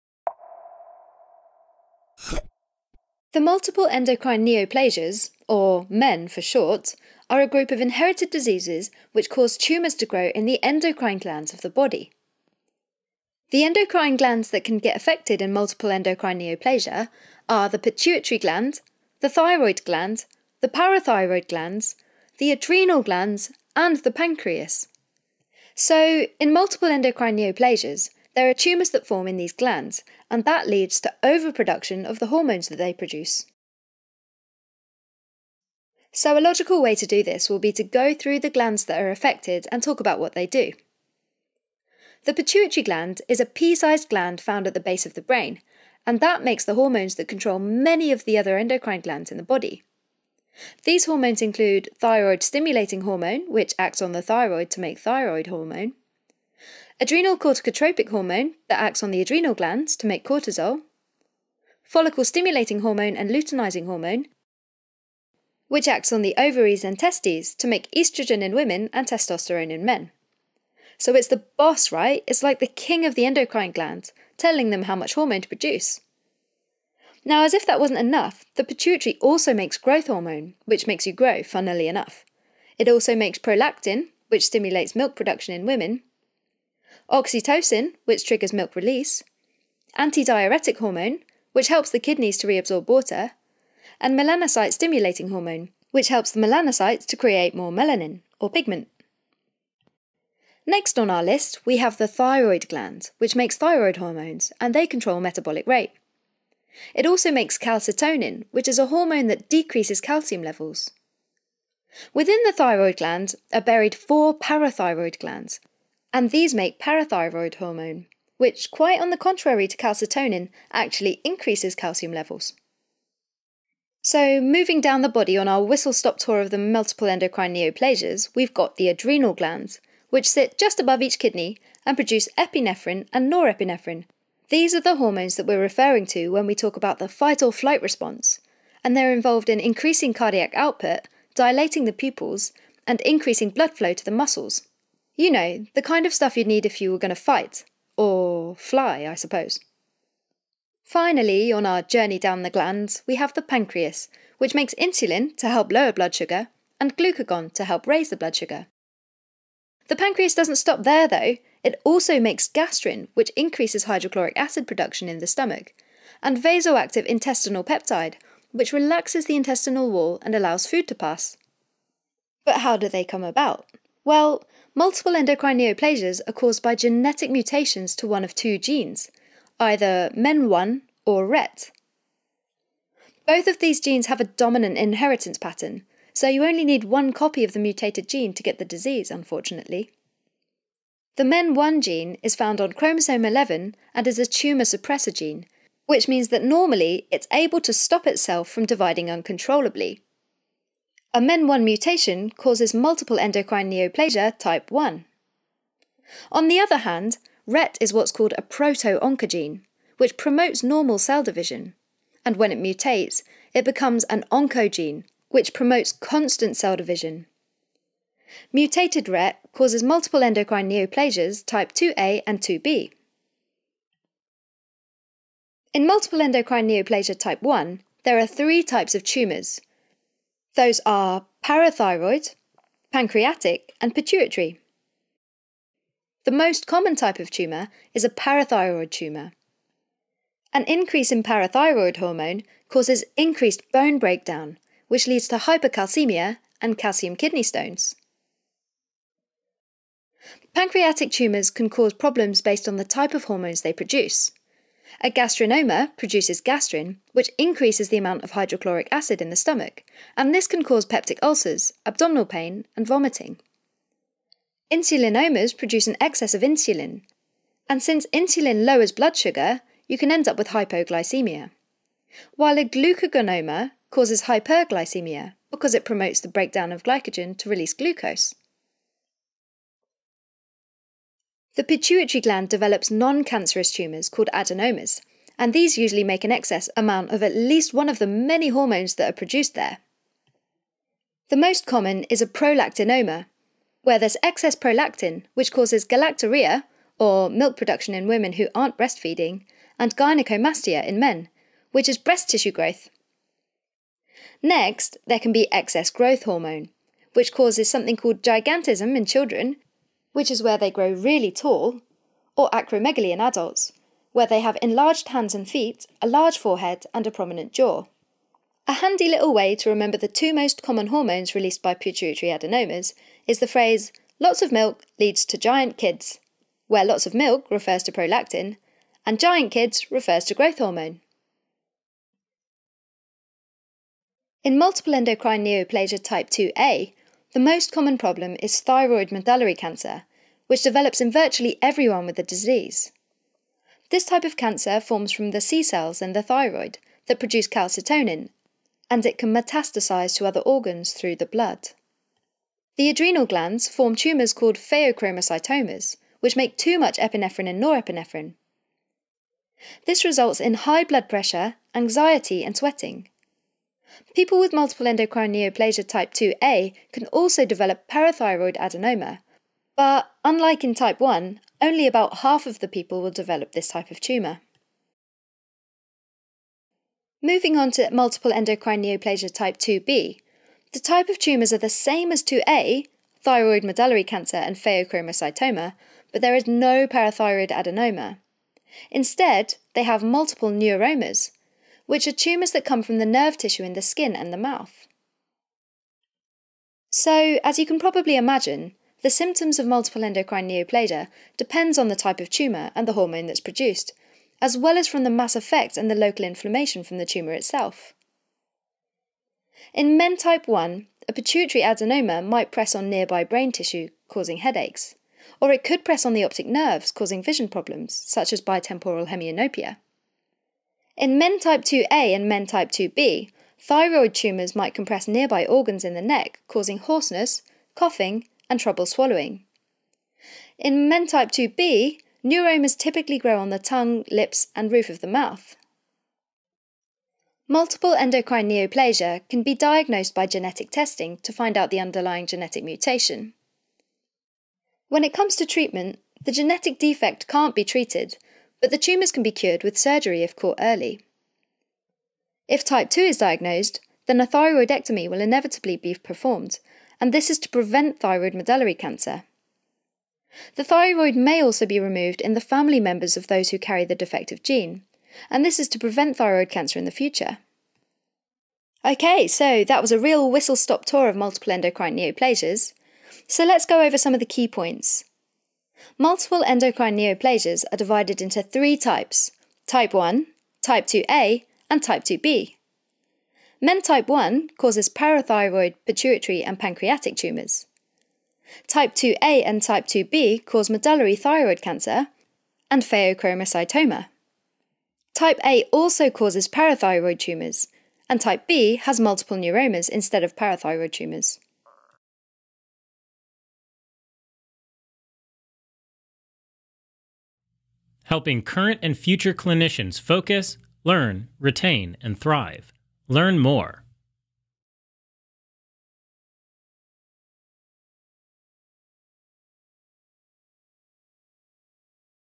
Multiple endocrine neoplasia - causes, symptoms, diagnosis, treatment, pathology
What is multiple endocrine neoplasia? MEN, for short, are a group of inherited diseases which cause tumors to grow in the endocrine glands of the body.